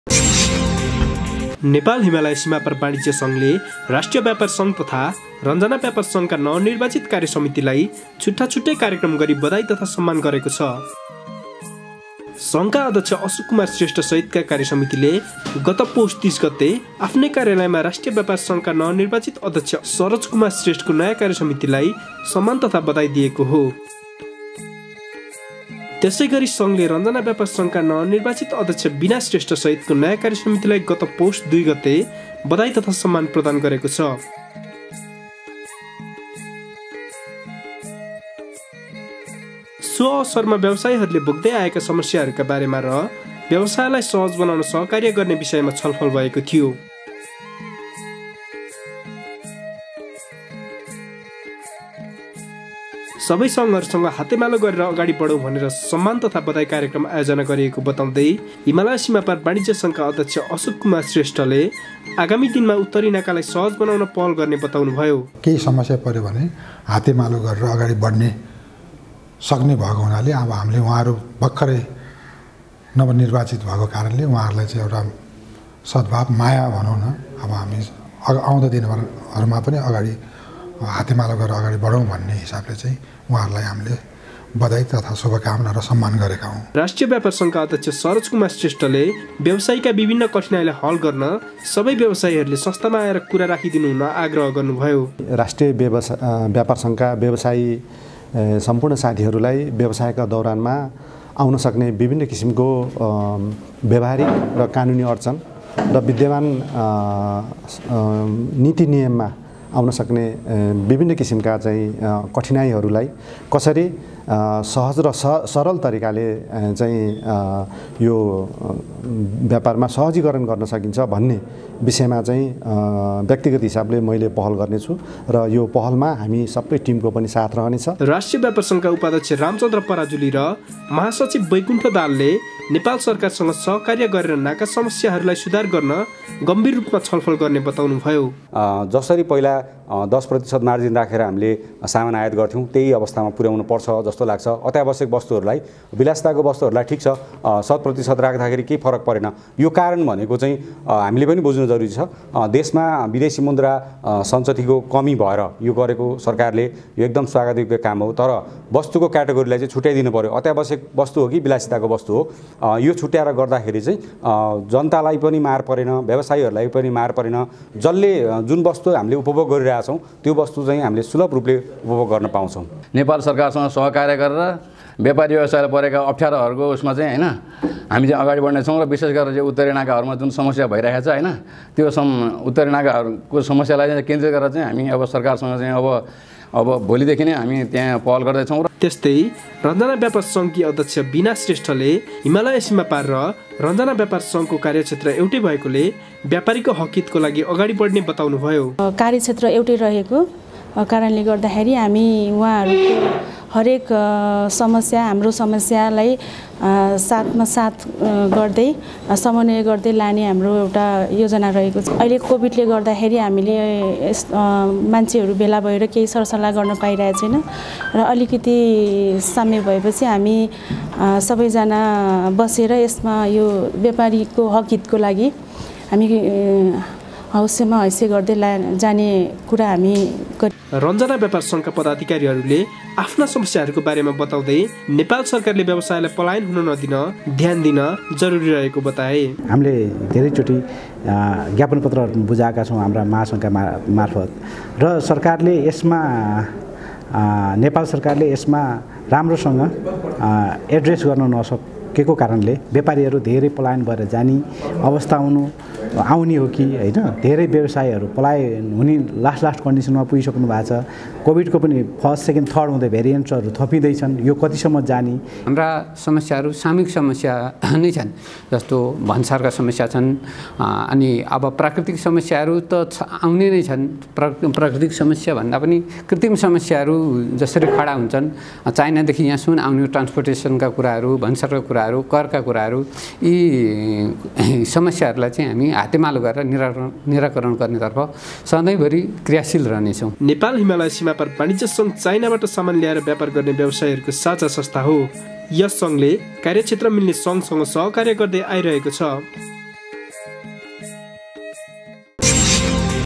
राष्ट्रिय व्यापार संघ र रञ्जना व्यापार संघलाई हिमालय सीमापारको स्वागत तथा सम्मान ...
Rojgar Media Pvt. Ltd.
Kamalpokhari, Kathmandu, Nepal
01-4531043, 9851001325